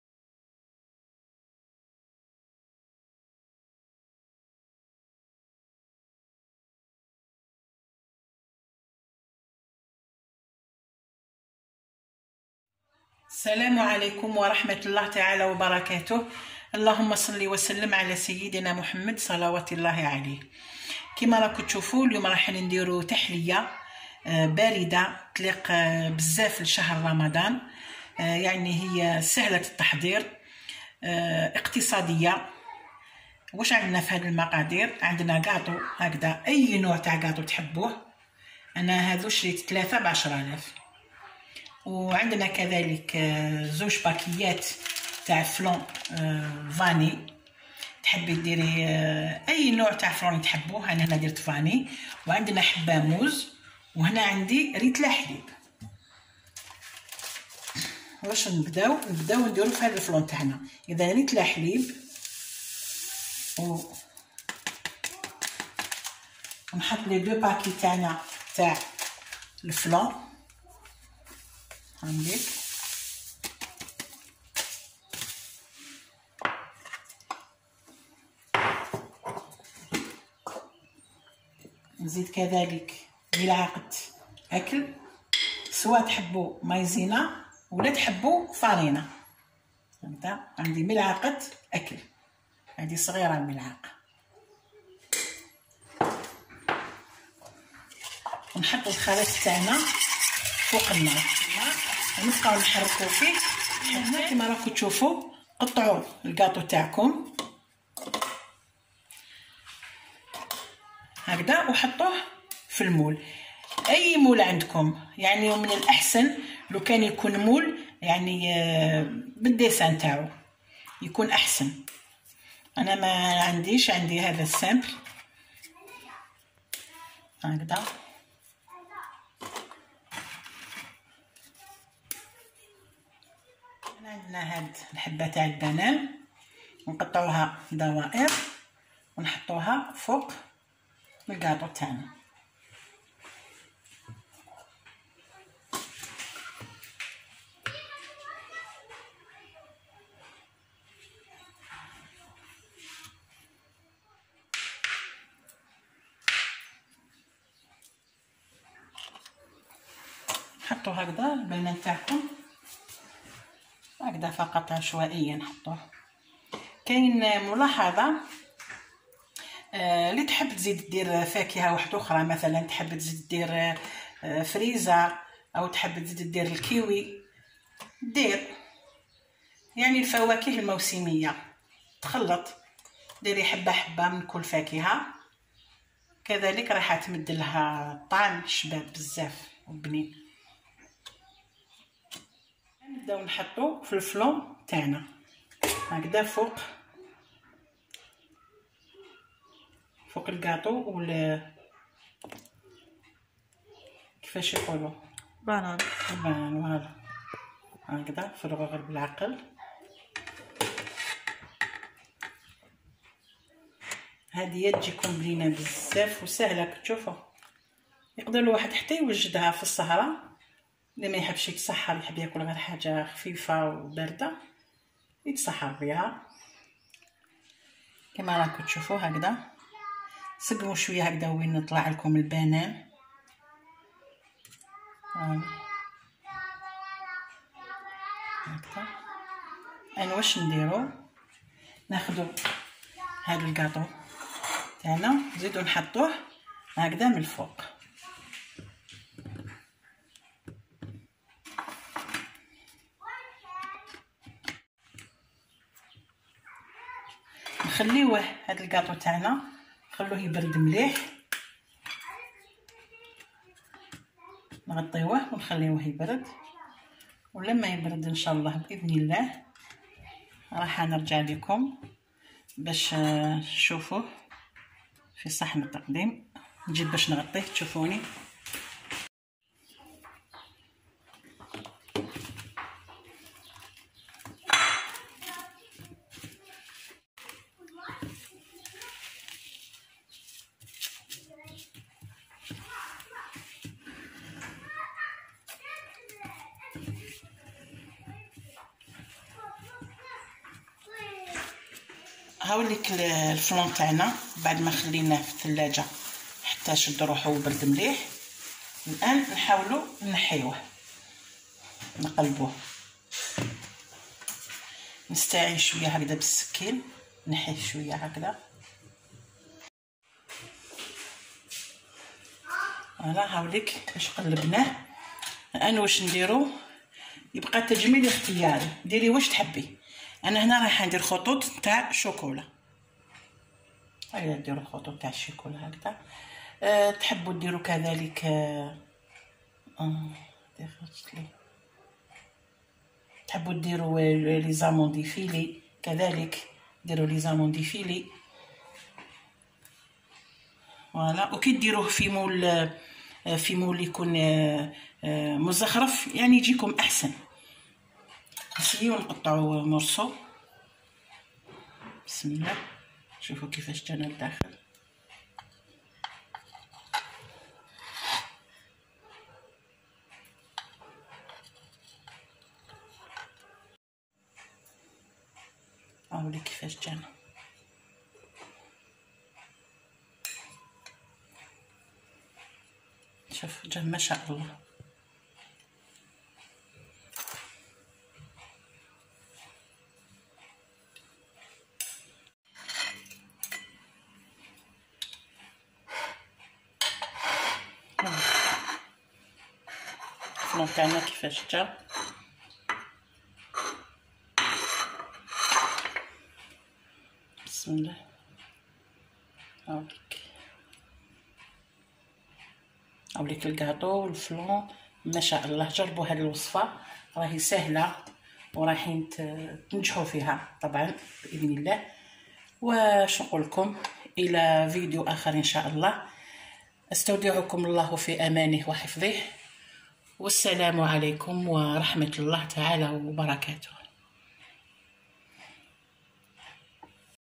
رمضان كريم#تحلية باردة تنحي علي الخاطر سهلة ماترطيهاش
تحلية بالموز والفلان
تحلية باردة

حبة موز لترا حليب
2علب فلان فاني
ملعبة كبيرة فرينة
قاطو للقاعدة
للتزين 50غ شيكولاته ذائبة